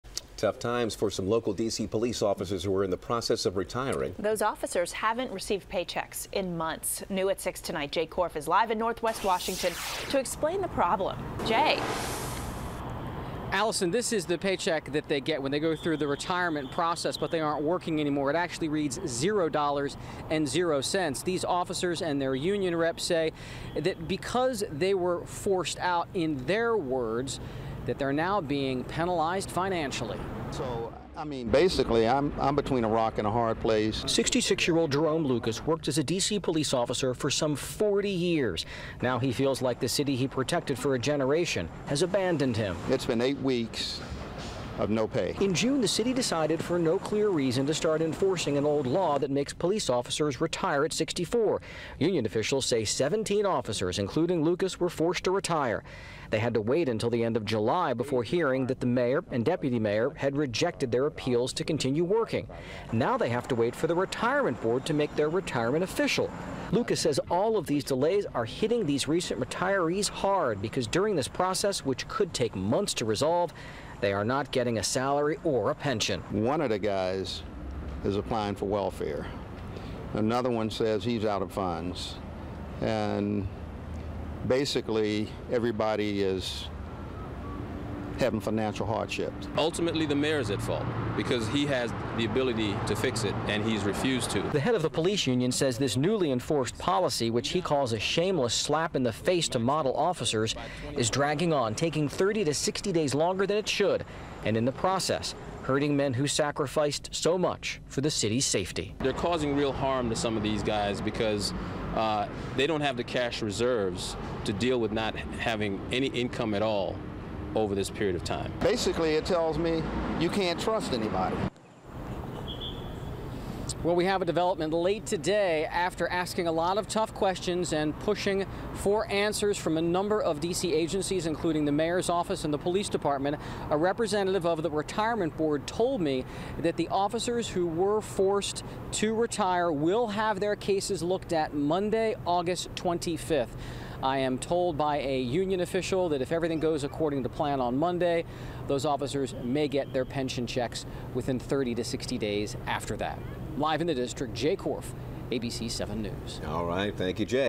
MPD officers forced to retire haven’t been paid in months
Some D.C. police officers who are in the process of retiring are facing tough times; they haven’t received paychecks in months.